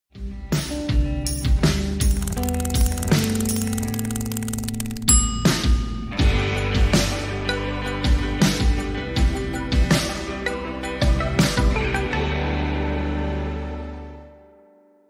THANK YOU! | One Day. One KU. 2021
KU’s fourth annual One Day. One KU. giving day brought in gifts benefiting everything from butterflies to Alzheimer’s disease research on its most successful giving day to date. The 24-hour event on Feb. 18 raised a total of $3,396,158 from 5,412 gifts to support the University of Kansas, including a $1 million gift to benefit the KU Alzheimer’s Disease Center.